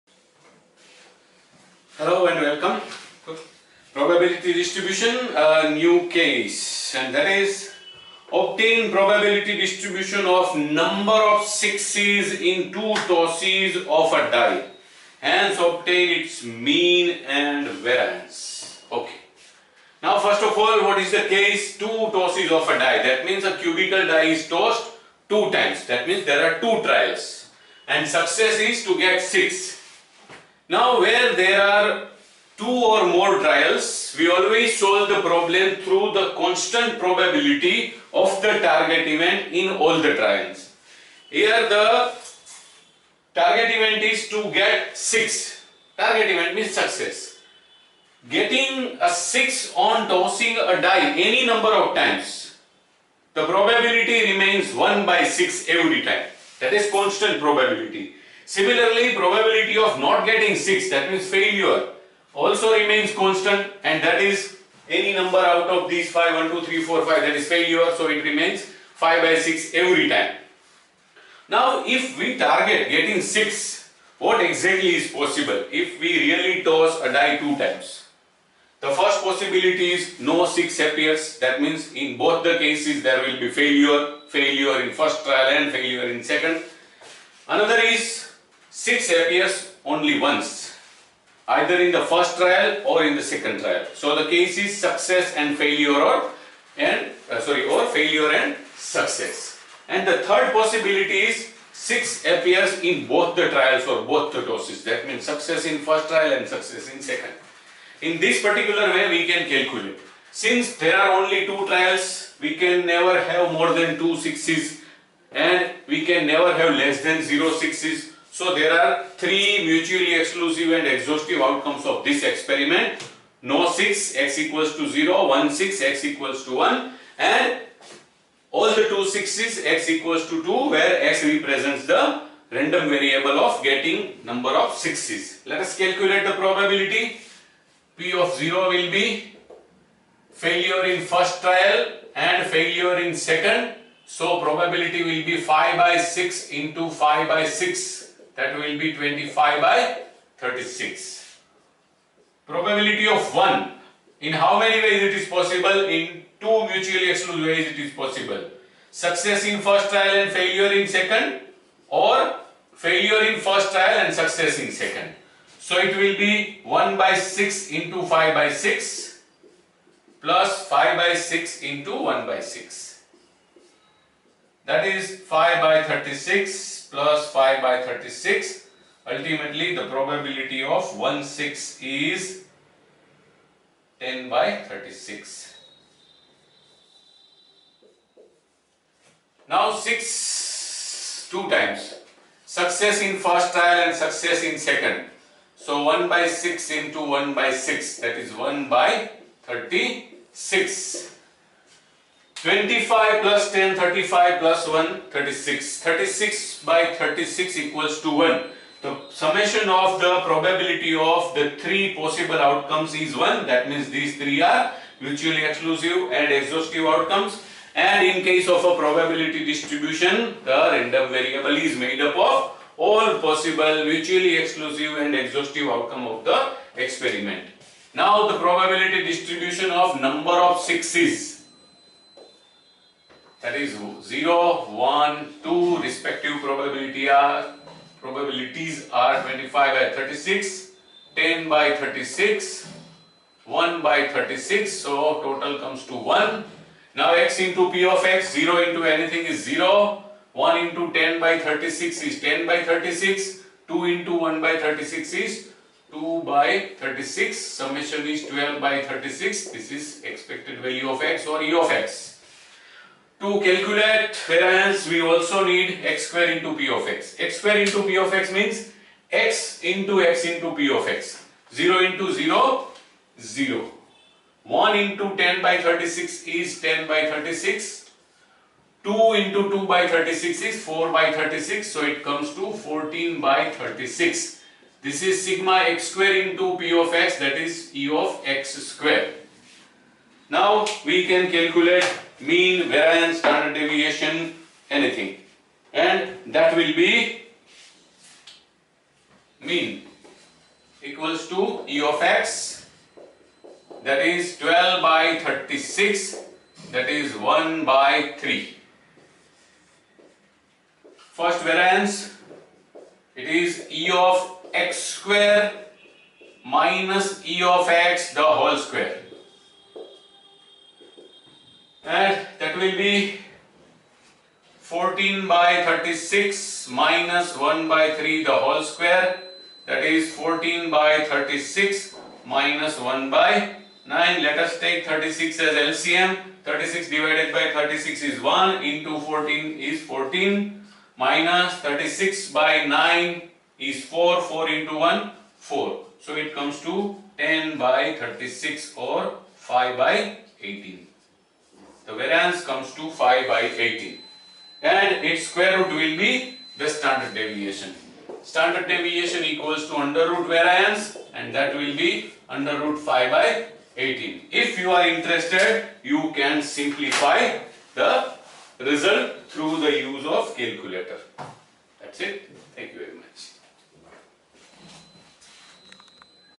Probability Distribution 15 Probability Distribution and Expected of No of 6s on 2 tosses of a die
Probability Distribution
Obtain probability distribution of ‘number of sixes’ in two tosses of a die. Hence obtain its mean and variance.

Probability Distribution - 15 - Basics